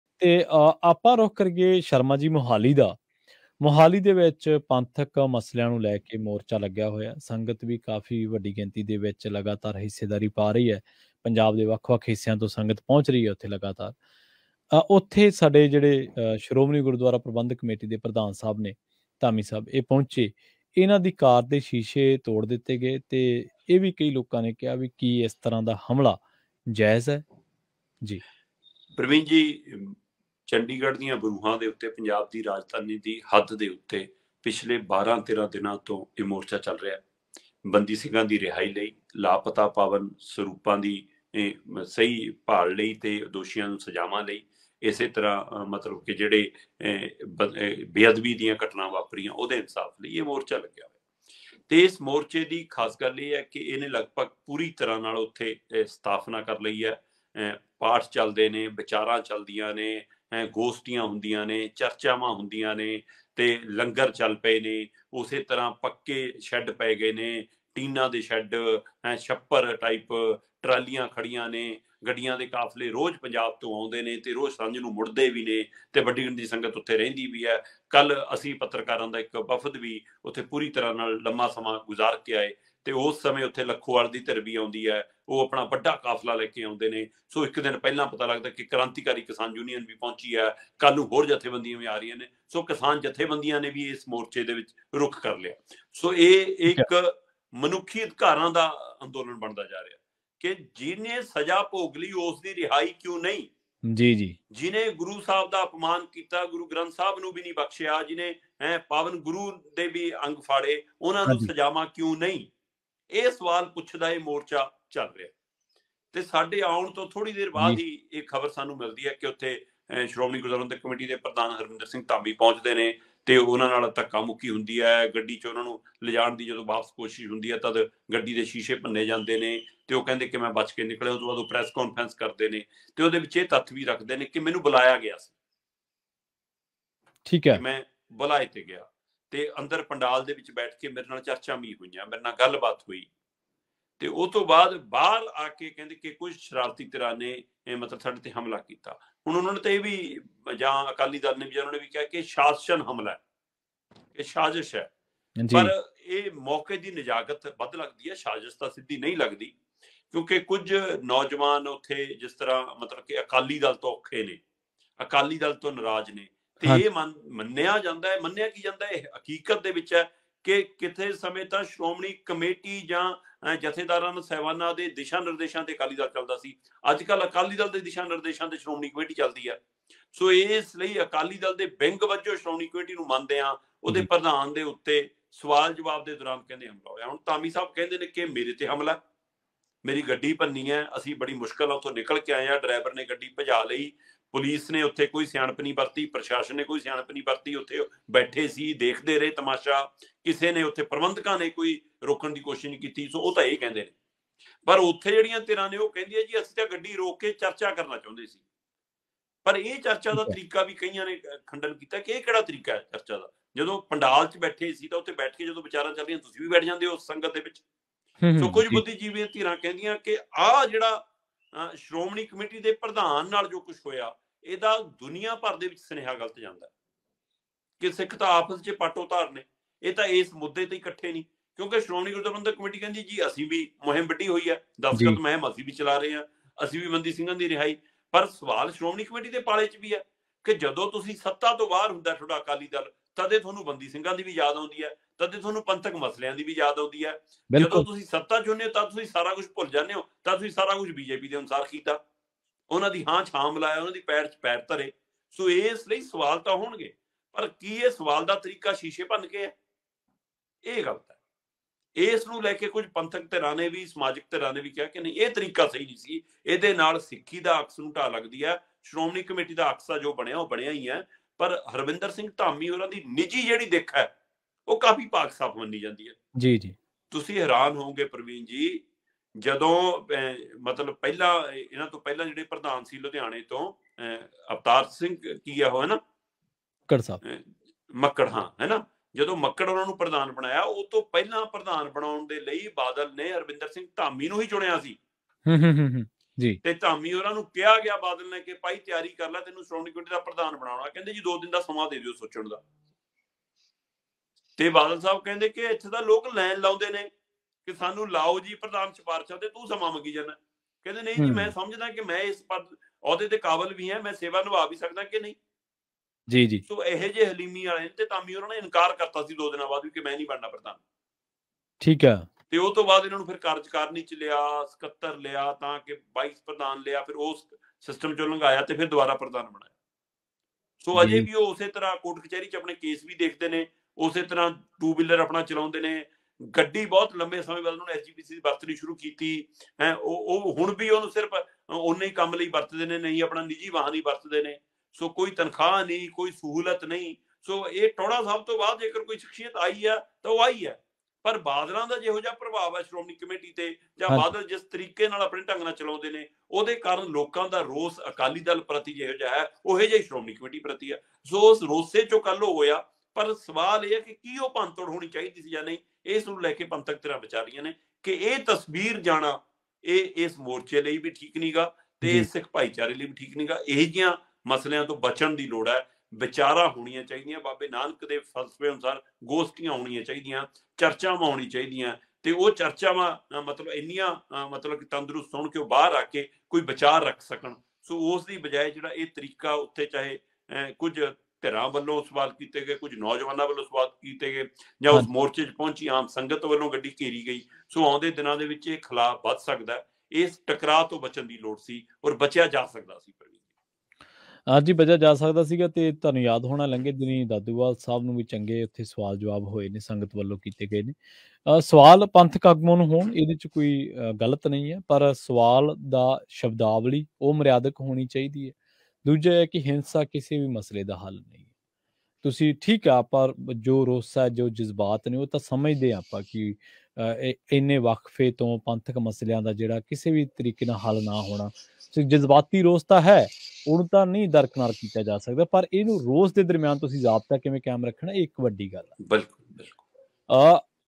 ਕੀ ਮੋਰਚੇ 'ਚ ਆਏ ਧਾਮੀ 'ਤੇ ਹਮਲਾ ਜਾਇਜ਼?
Attack on Dhami: Car attacked, SGPC chief Harjinder Singh Dhami escapes unhurt. When Dhami sat in his car, his vehicle was attacked from front and rear side. However, he managed to escape unhurt.

Breaking news in Punjabi first on Punjabi Radio USA. Latest news, live update news, read most reliable Punjabi news on Punjabi Radio USA.